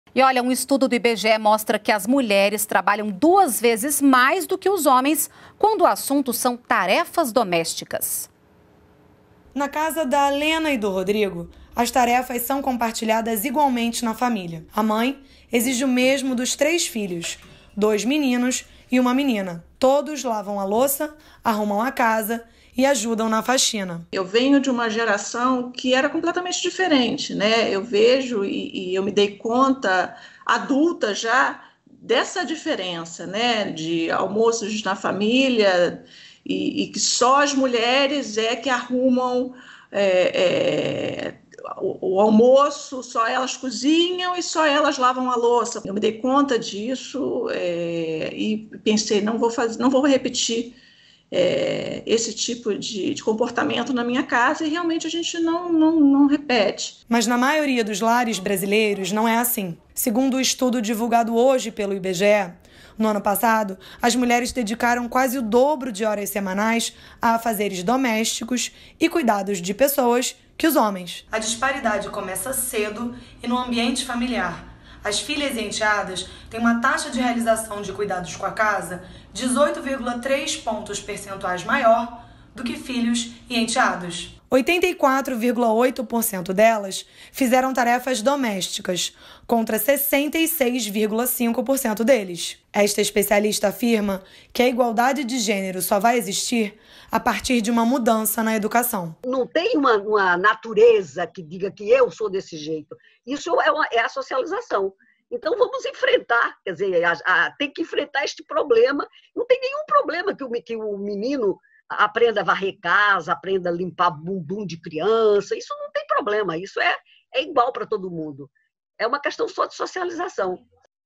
Pesquisa mostra que mulheres fazem maior parte do serviço doméstico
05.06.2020 - As mulheres trabalham duas vezes mais do que os homens quando o assunto são tarefas domésticas. O levantamento foi divulgado pelo IBGE.